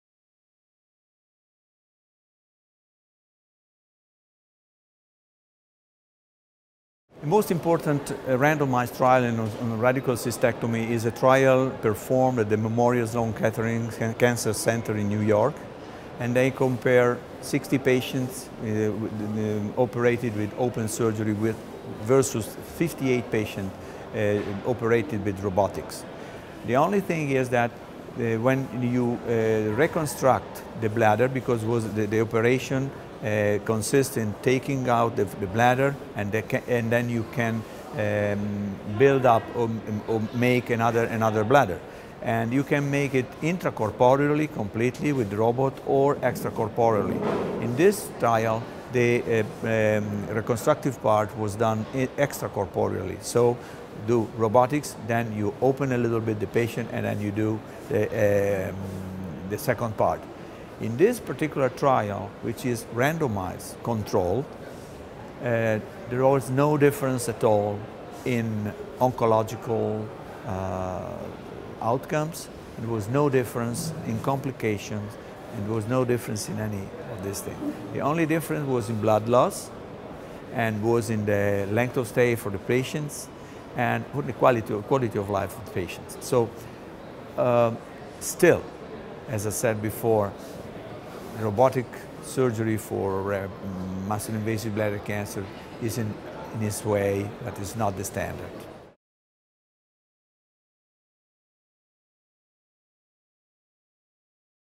Is robotic surgery better for muscle invasive bladder cancer?
Maurizio Brausi, MD, from the AUSL Modena, B. Ramazzini Hospital, Modena, Italy, discusses clinical trials of robotic surgery in radical cystectomy at the European Association of Urology (EAU) conference 2017 in London, UK. According to Prof. Brausi, the most important randomized trial in robotic surgery for radical cystectomy is a Phase III study carried out at the Memorial Sloan Kettering Cancer Center in New York, NY (NCT01076387). Prof. Brausi summarizes the results of the trial, and concludes that robotic surgery for muscle invasive bladder cancer shows promise but is not yet the standard.